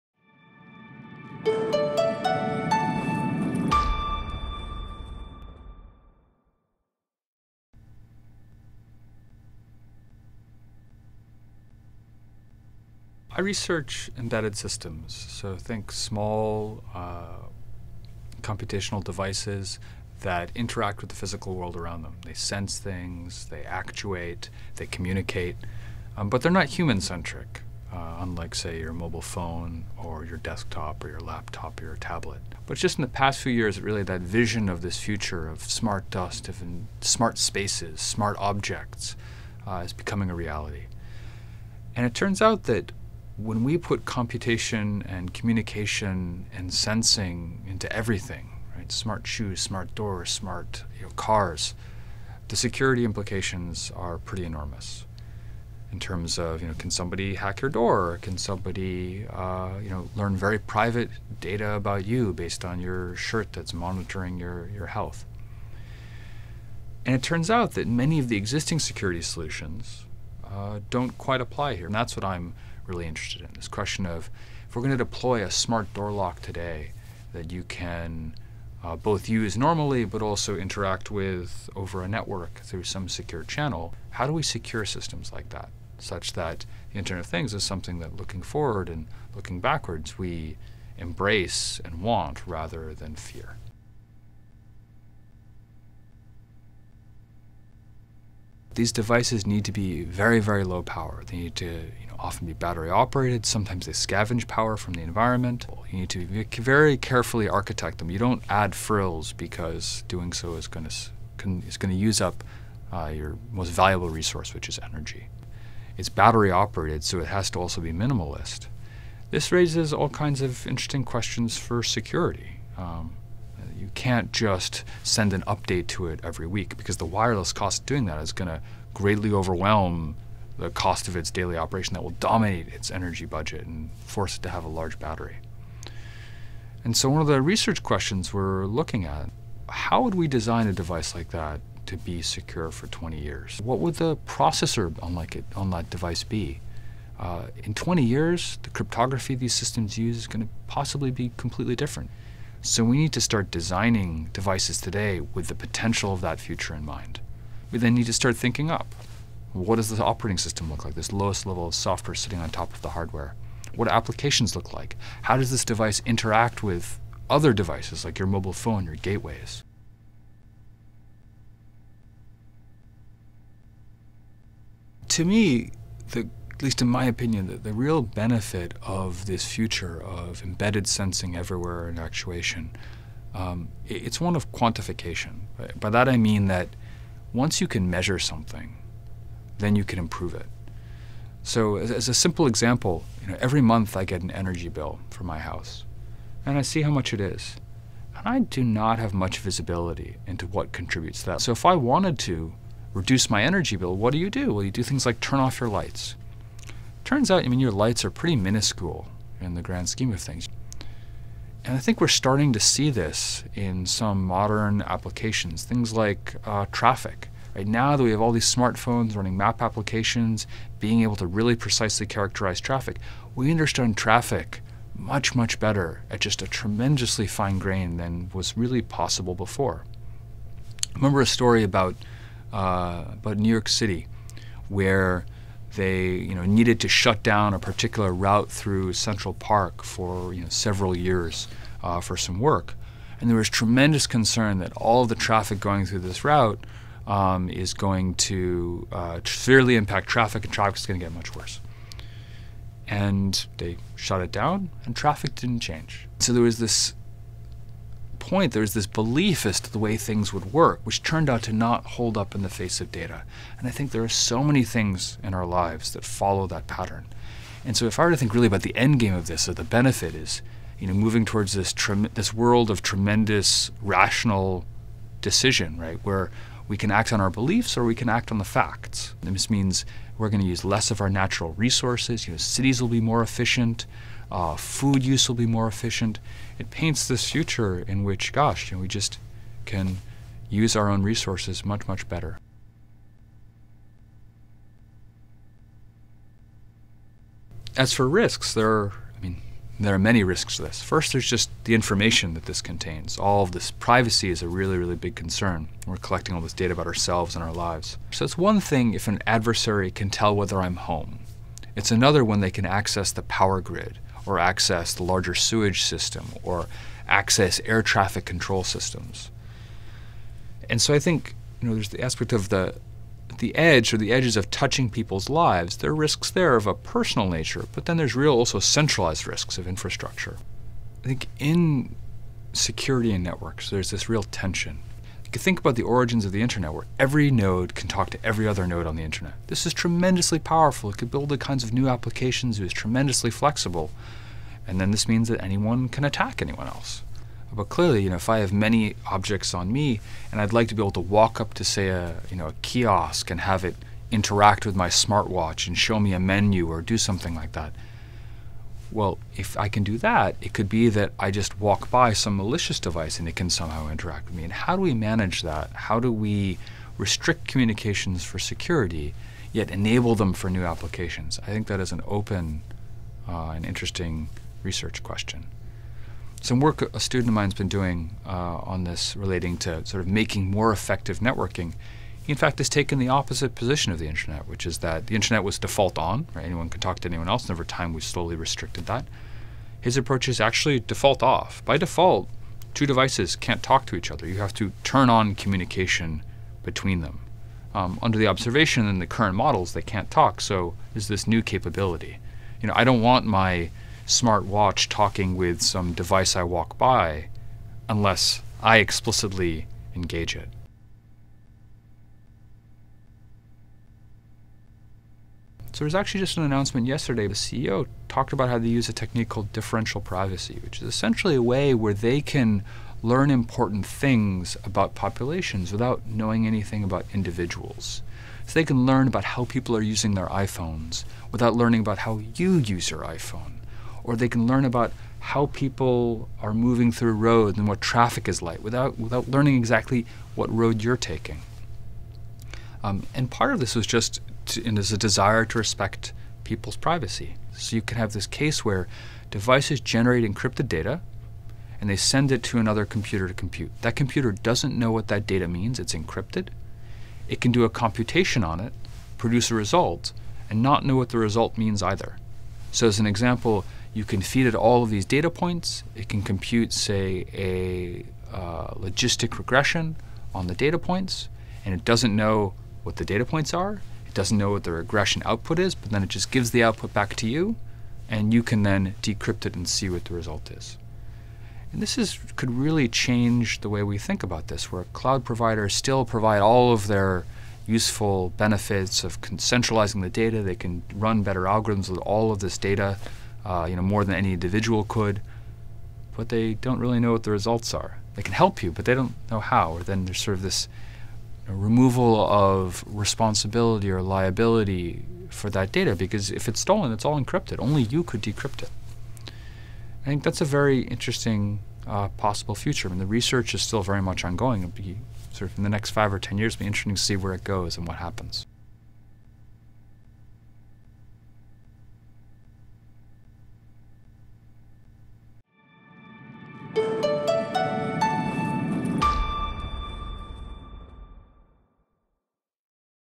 Phil Levis: Will we embrace or fear the Internet of Things?
Phil Levis offers a few thoughts on the promise and the perils of the Internet of Things. He is an Assistant Professor of Electrical Engineering and of Computer Science at Stanford and is helping to design the next generation of small, interconnected devices that will fuel the Internet of things.

“I research embedded systems, so think small computational devices that interact with the physical world around them unlike, say, your mobile phone or your desktop or your laptop or your tablet. Just in the past few years really that vision of this future of smart dust, and of smart spaces, and smart objects, it's becoming a reality. And it turns out that when we put computation and communication and sensing into everything, right, smart shoes, smart doors, smart cars, the security implications are pretty enormous.”